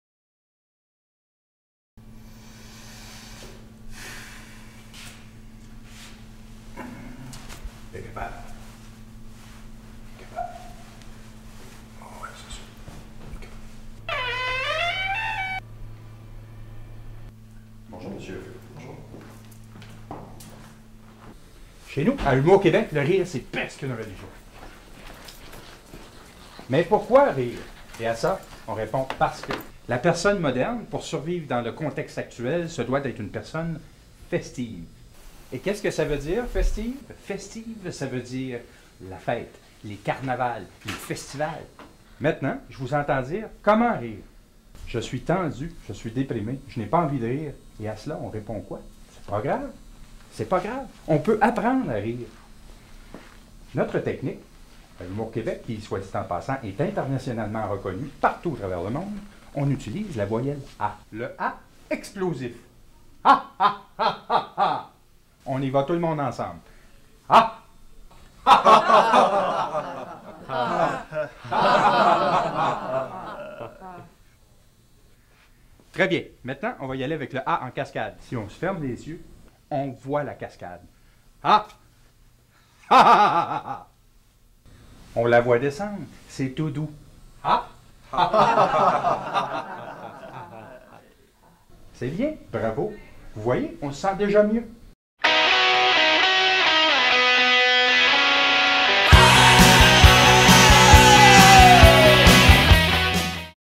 Reproduction d'une scène de "L'âge des ténèbres" de Denys Arcand
Projet de fin de session pour le cours Techniques de création dans le programme Arts et lettres: profil Cinéma en décembre 2009.